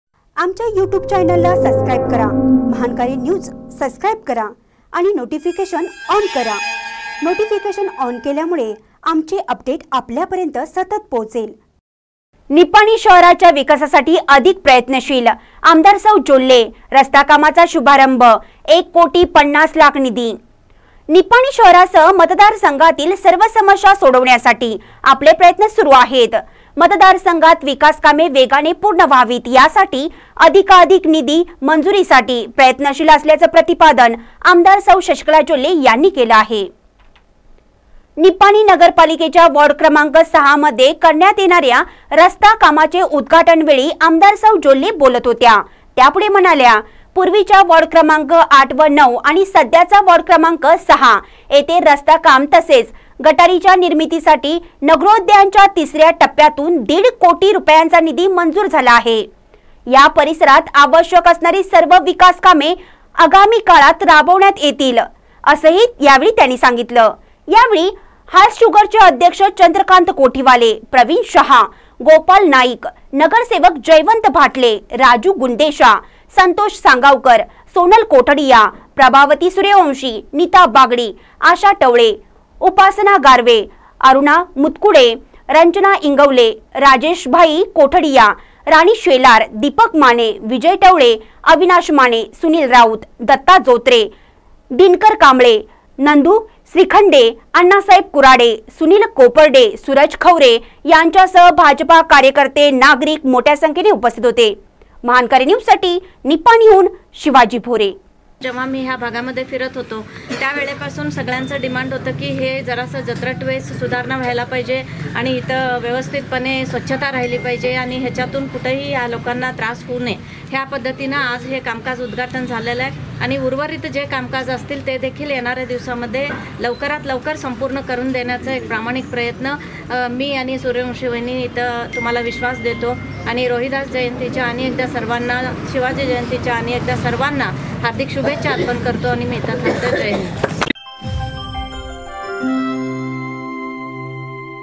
निपाणी शहराच्या विकासासाठी अधिक प्रयत्नशील - आ. सौ जोल्ले
निपाणी शहराच्या विकासासाठी अधिक प्रयत्नशील - आ. सौ जोल्ले : रस्ता कामाचा शुभारंभ, १ कोटी ५० लाख निधी.